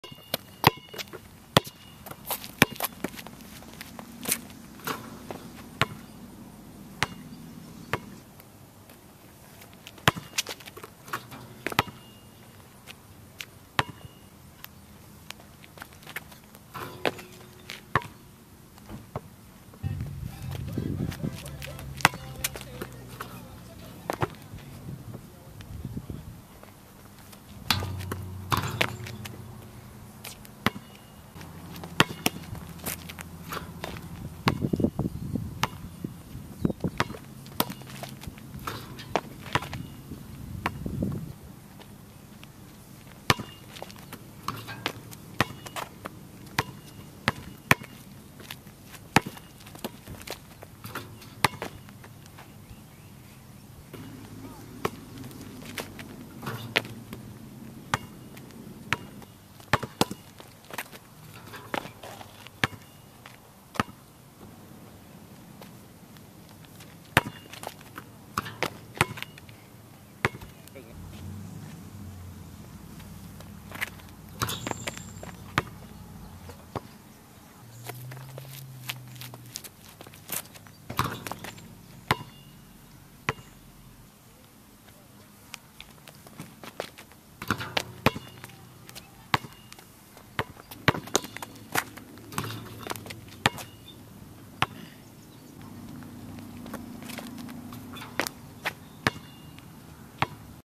Whitetop Park Dunks
From summer of 2011. Just out at the park throwing down some dunks.